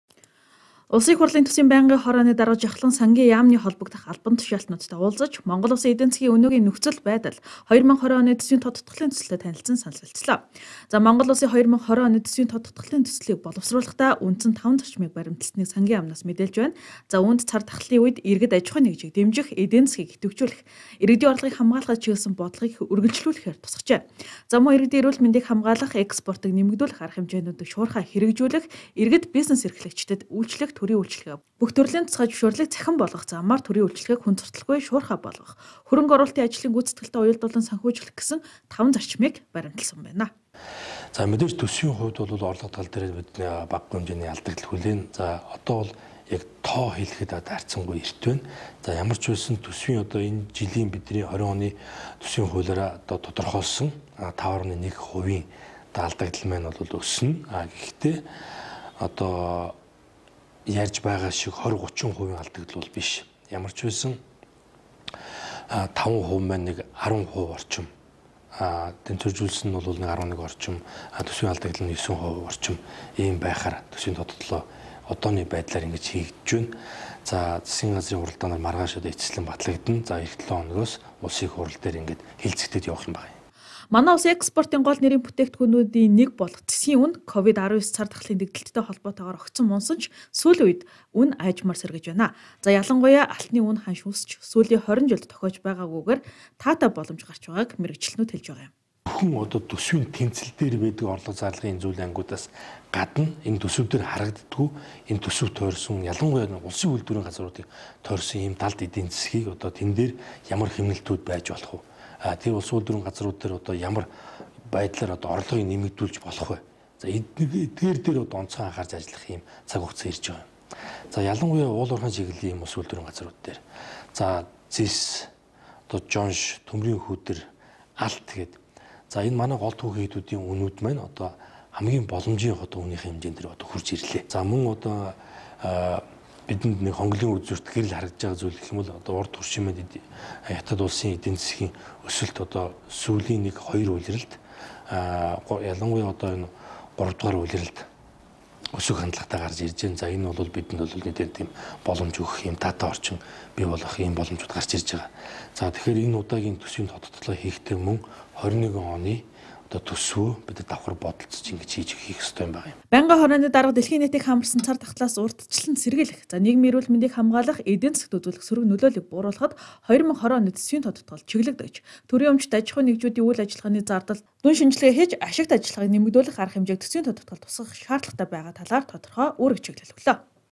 2020 оны төсвийн тодотголын асуудлаар Сангийн яамныхантай санал солилцлоо
Төсвийн байнгын хорооны дарга Б.Жавхлан 2020 оны төсвийн тодотголын асуудлаар Сангийн яамныхантай санал солилцлоо/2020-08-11/